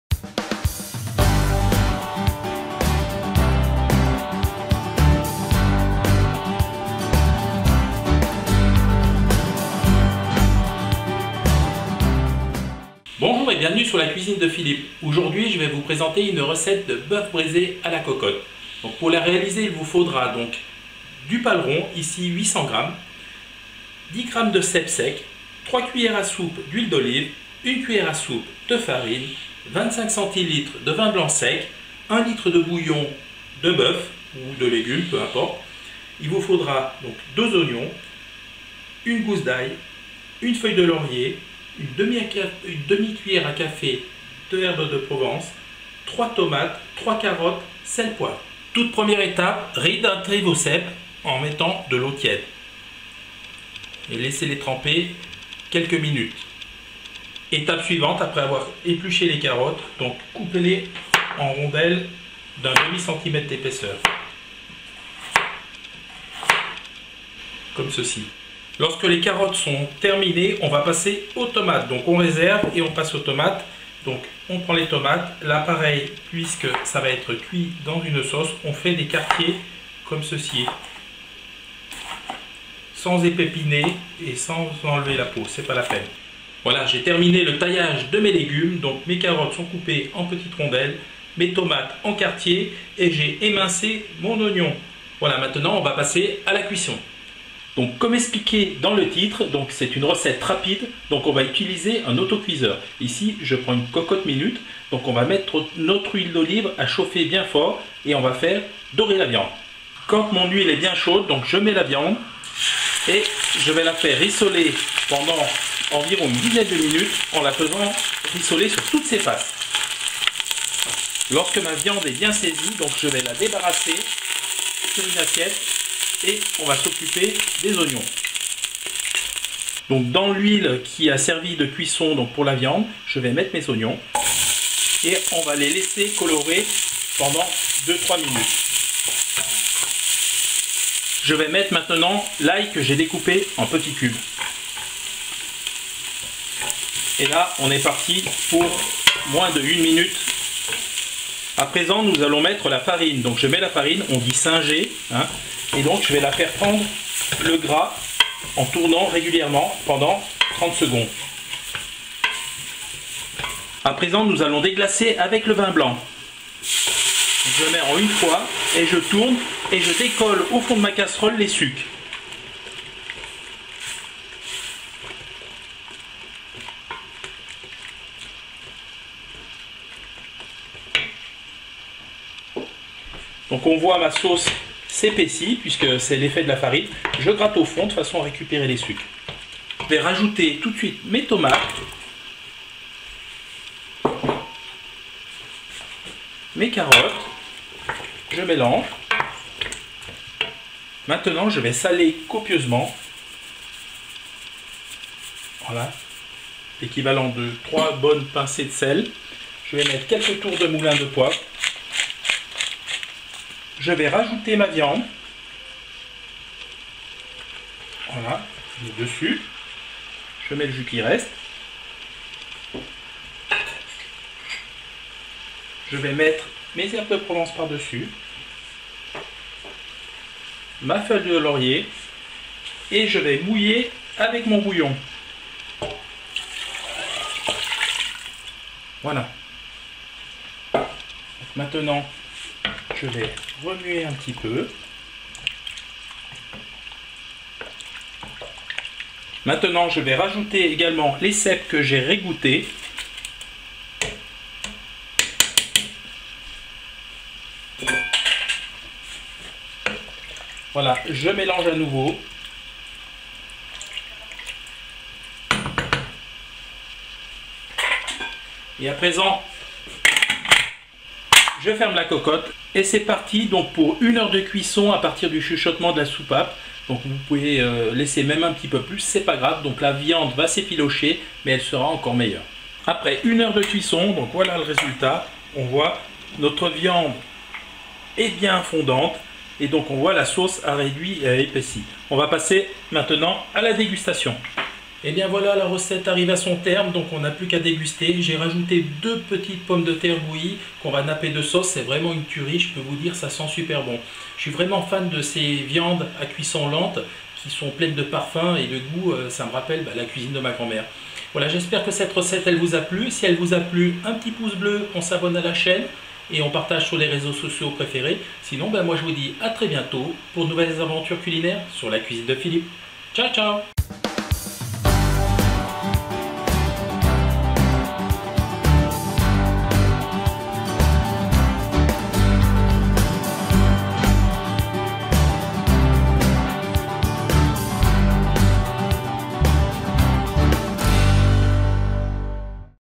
Boeuf braisé à la cocotte
Le boeuf braisé est une recette traditionnelle qui est quelque peu délaissée actuellement comme toutes les viandes bouillies ou à cuisson longue. Souvent, le temps de mijotage rebute les cuisiniers et les fait se tourner vers les viandes grillées...
Personnellement, je suis un fan inconditionnel du boeuf braisé que j'ai modernisé avec l'utilisation d'un autocuiseur. Ainsi le temps de cuisson est au moins divisé par deux ! Avec quelques cèpes secs, la sauce prend du tonus et nous donne envie d'y retourner. Cependant, vous pouvez tout à fait mettre d'autres champignons, le résultat sera au rendez-vous. J'ai testé avec des trompettes des morts c'est vraiment délicieux.
Bref, je vous invite à vous lancer dans l'aventure en testant cette recette de boeuf braisé qui n'a que des qualités, certes gustatives mais aussi économiques. A vos fourneaux !

Ingrédients :
800 gr de boeuf à braiser 800 gr [ex : paleron]
25 cl de vin blanc sec
2 petits ou 1 gros oignons
1 gousse d'ail
3 carottes
3 tomates
3 cuillères à soupe d'huile d'olive
1 cuillère à soupe de farine
10 gr de cèpes secs
1/2 cuillère à café d'herbes de Provence
1 feuille de laurier
1 litre de bouillon de boeuf [2 tablettes dans 1l d'eau ]
Sel/Poivre

Temps de cuisson : 1h00 à compter du chuchotement de la soupape.

vous pouvez soutenir la chaîne en faisant